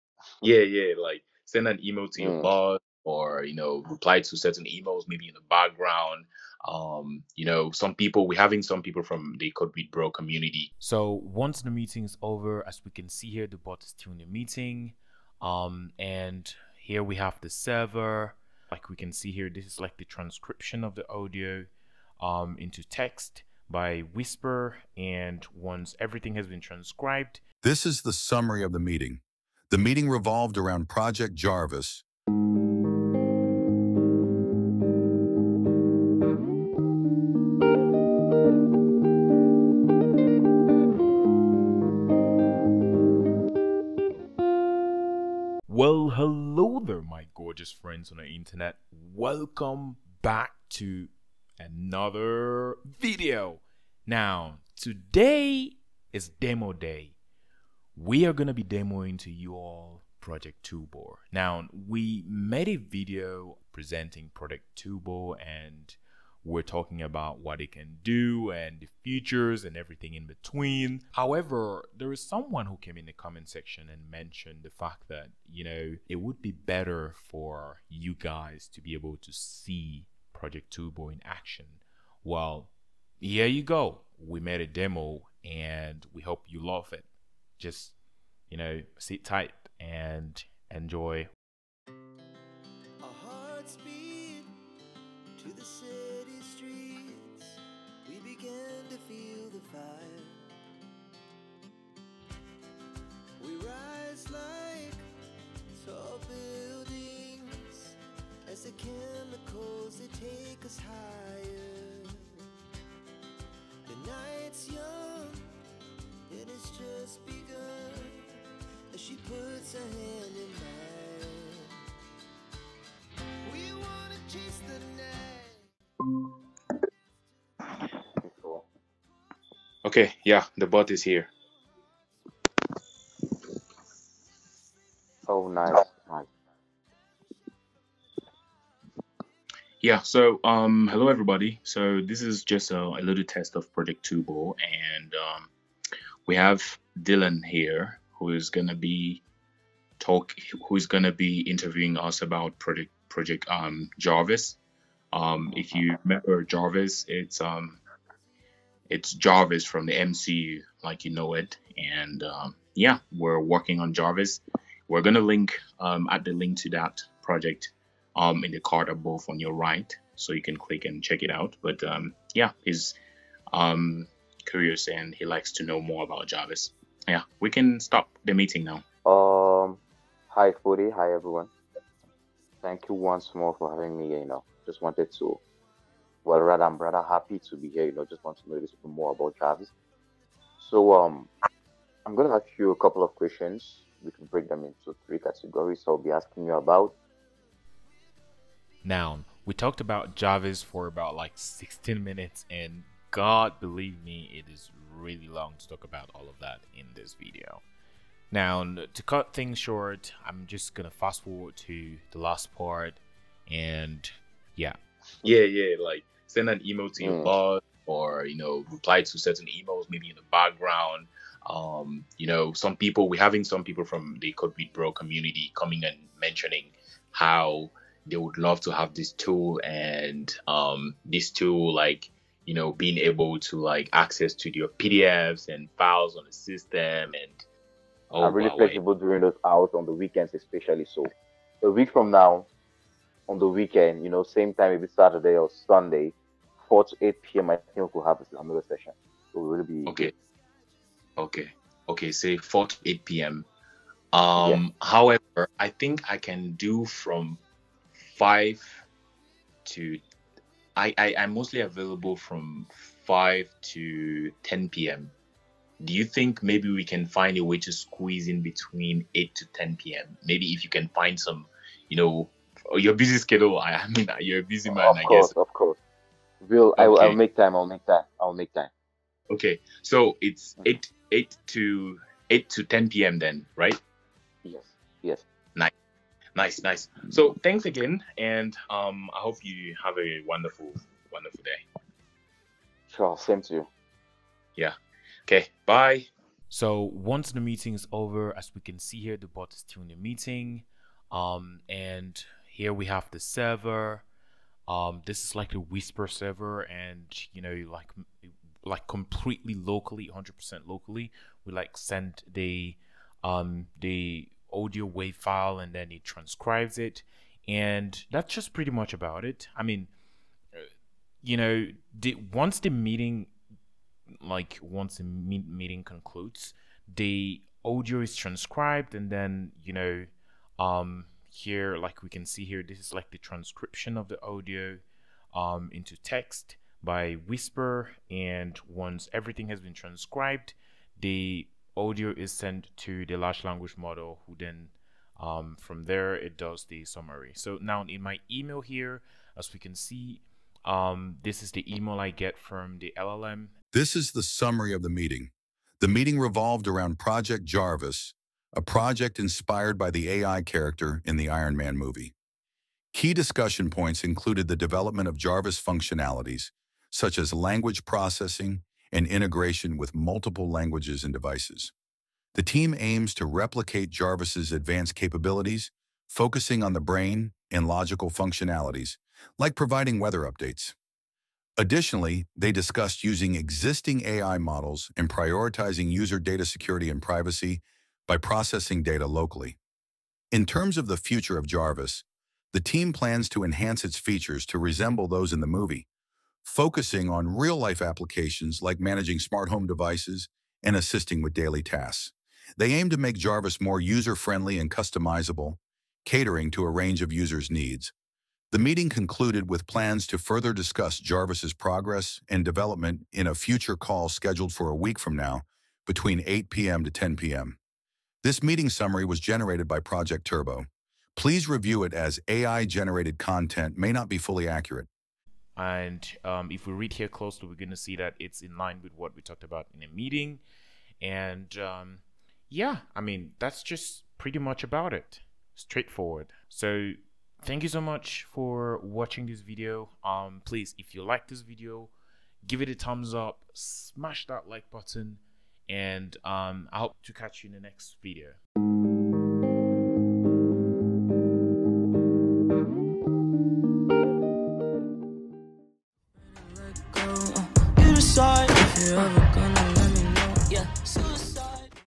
Your Meeting Minutes with AI for Free: note-taking made easy!
What's up amazing people on the internet, Here is a quick video show-casting how project-turbo would actually perform in a real life setting.